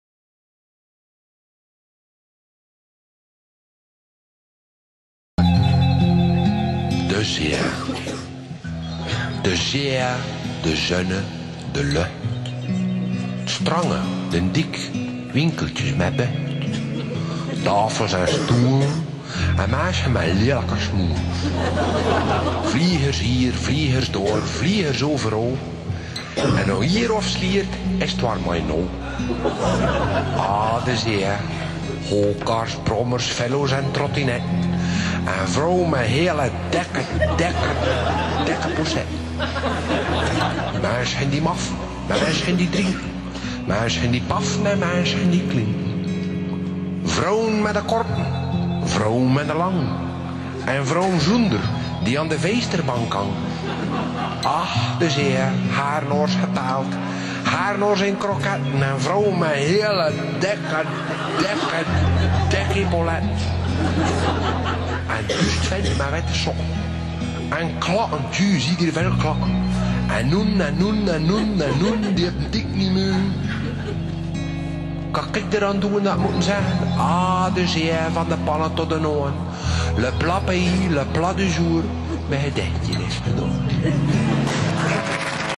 De Zee
De zee, de zon, de lucht. Het strand, de dijk, winkeltjes met bucht. Tafels en stoelen, mensen met lelijke smoelen. Vliegers hier, vliegers daar, vliegers overal. En als je hieraf sliert is het warm aan je hol. Ahh, de zee... go-karts, brommers, velo's en trottinetten. .
Presented by: Delta Pipa Studios 2008